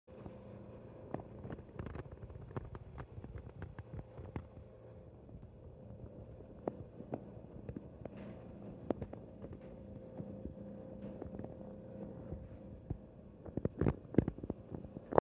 Naughty abyssinian cat made a mess
Cat made a complete mess in the kitchen. Her food ended up on the floor and even on the wall. I don't know how this is possible but she is very talented.